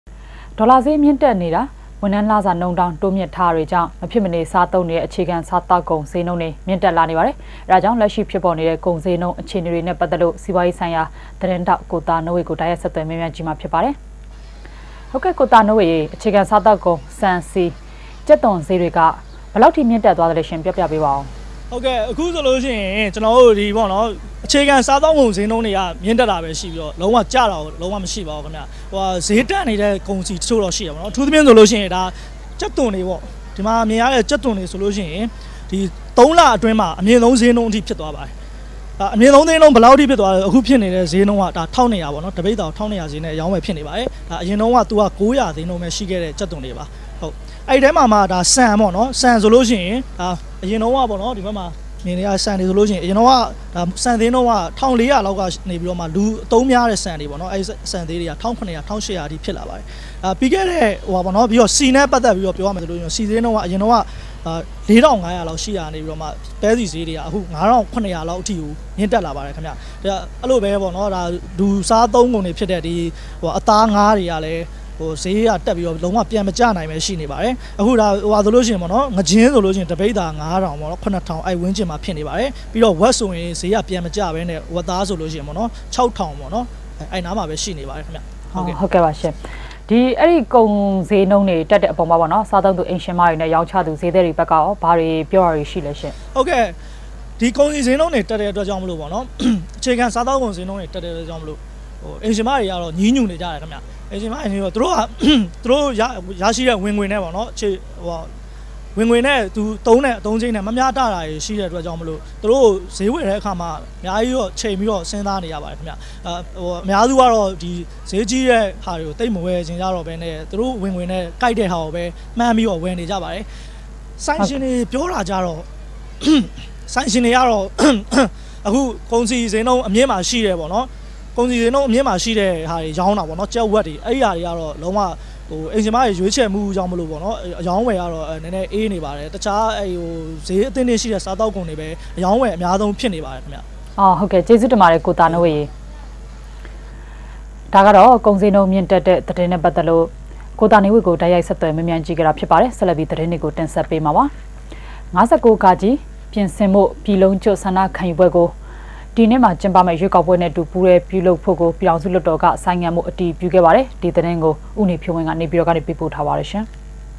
DVB - အေျခခံစားေသာက္ကုန္ေစ်းႏွဳန္းေတြ ျမင့္တက္
DVB TV - ေဒၚလာေစ်း ျမင့္တက္ေနတာ ၀န္ထမ္းလစာႏွုန္းထား တုိးျမင့္ထားတာေတြေၾကာင့္ မျဖစ္မေန စားသံုးေနတဲ့ အေျခခံ စားေသာက္ကုန္ ေစ်းႏွုန္းေတြ ျမင့္တက္လာေနပါတယ္။ အဲဒါေၾကာင့္ လက္ရွိျဖစ္ေပၚေနတဲ့ ကုန္ေစ်းႏႈန္း အေျခအေနေတြနဲ႔ ပတ္သက္လို႔ စီးပြားေရးဆိုင္ရာ သတင္းေထာက္ ကိုသာႏိုးေ၀ကို တိုက္ရိုက္ ေမးျမန္းၾကည့္မွာ ျဖစ္ပါတယ္။